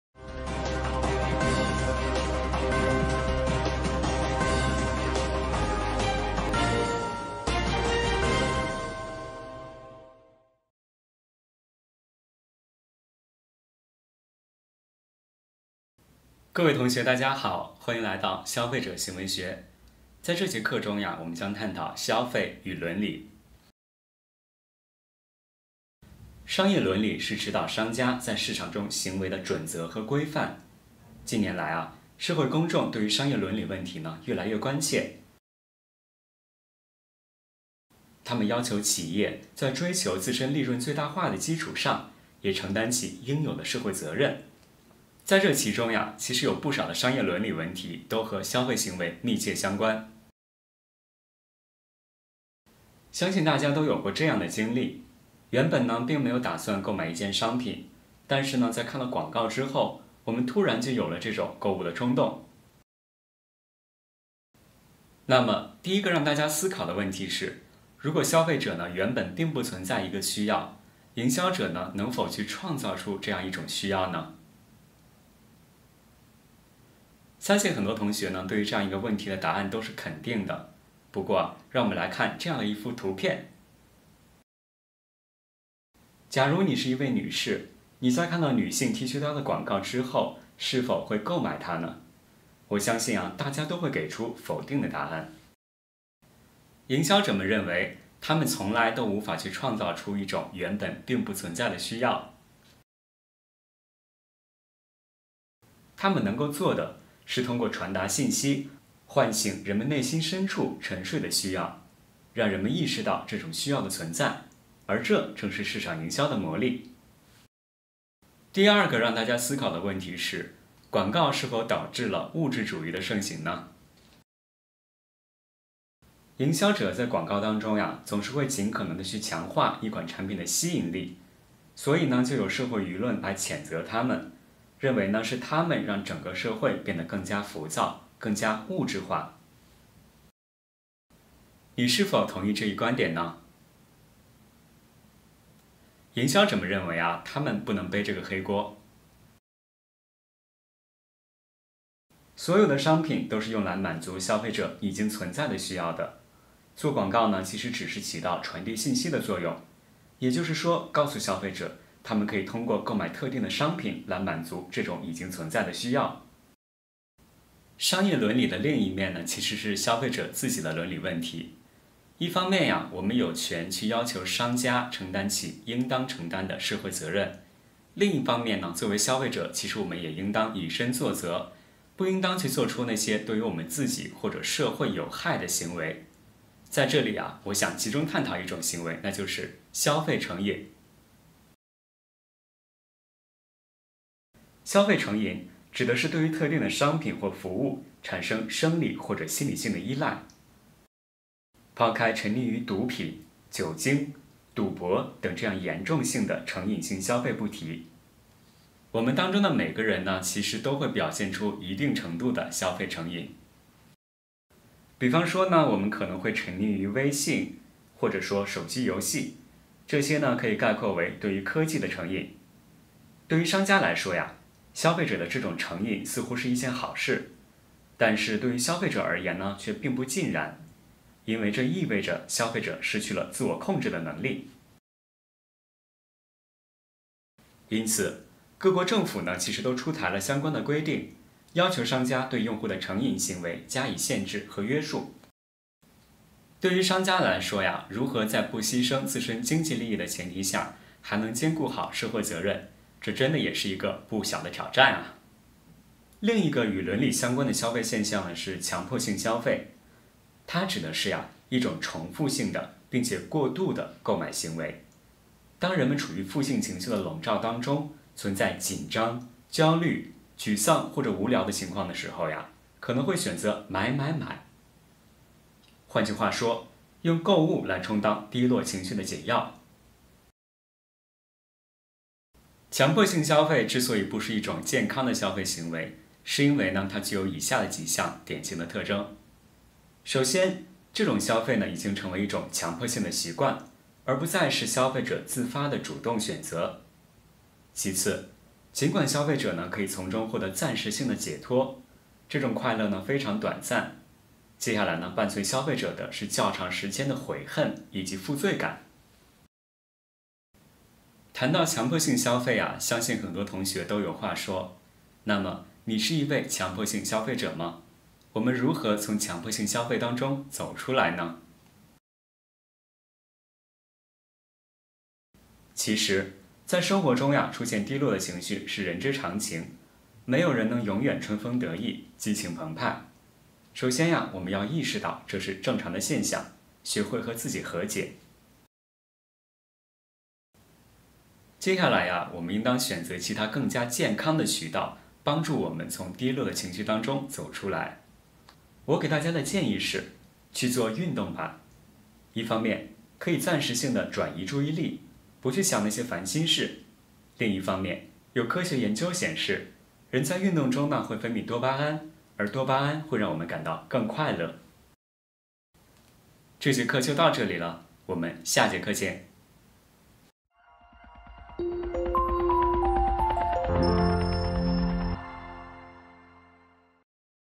消费者行为学 第一讲消费者行为学基础 消费与伦理
《消费者行为学》中，我们将深入探讨消费者决策、信息加工、外部与内部因素对消费者行为的影响。为了更深入的洞察复杂的消费行为，我们将系统介绍与消费者行为相关的一系列心理学、社会学和经济学知识。在此基础上我们将进一步探索数字营销的魔力，探讨数字科技与消费者行为之间的相互作用。 精品公开课